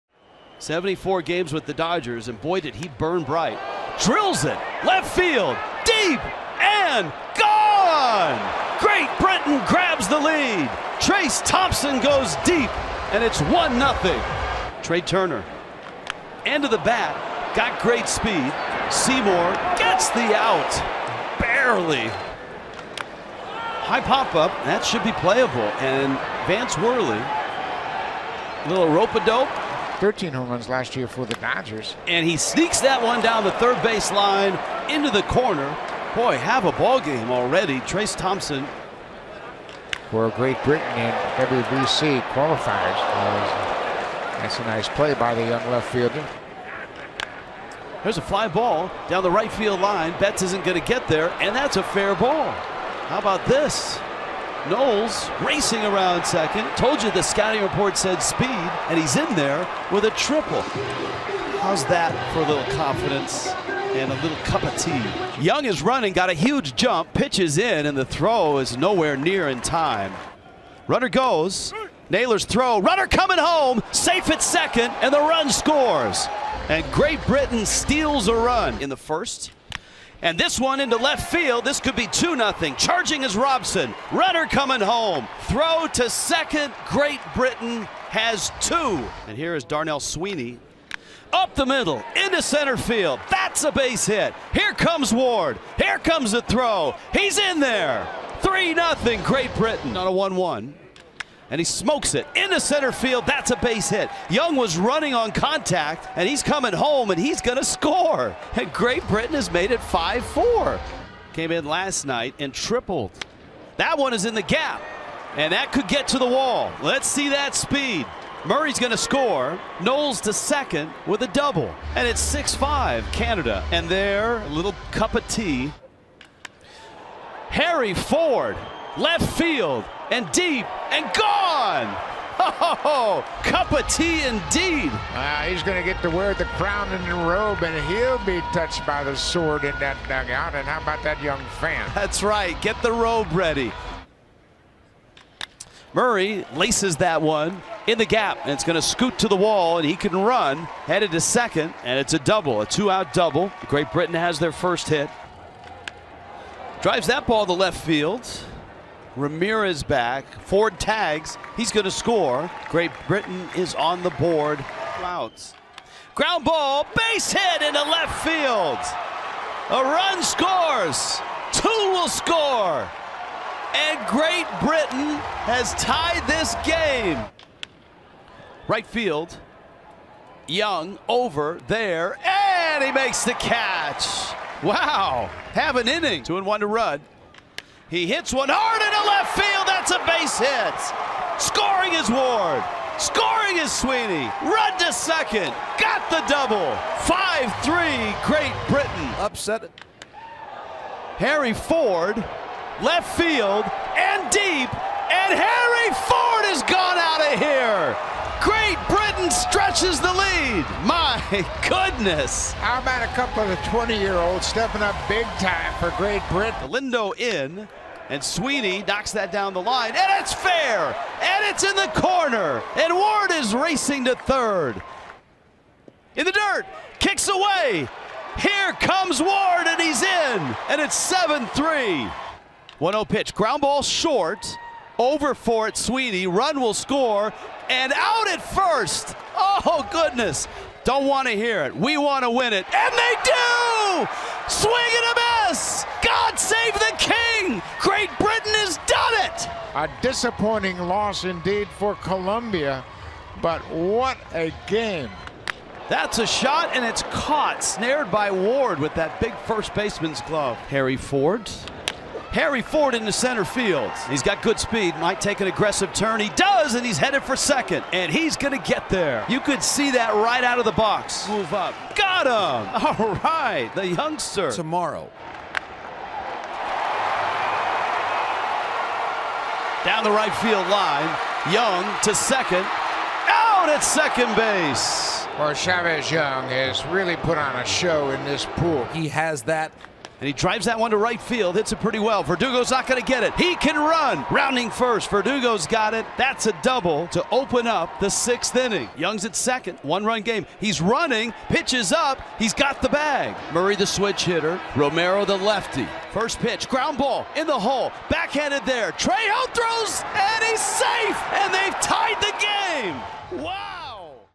MLB | Team Great Britain - 2023 WBC Highlights
Great Britain finished 1-3 in Pool C play.

0:00 Great Britain vs United States
1:05 Great Britain vs Canada
2:24 Colombia vs Great Britain
4:15 Great Britain vs Mexico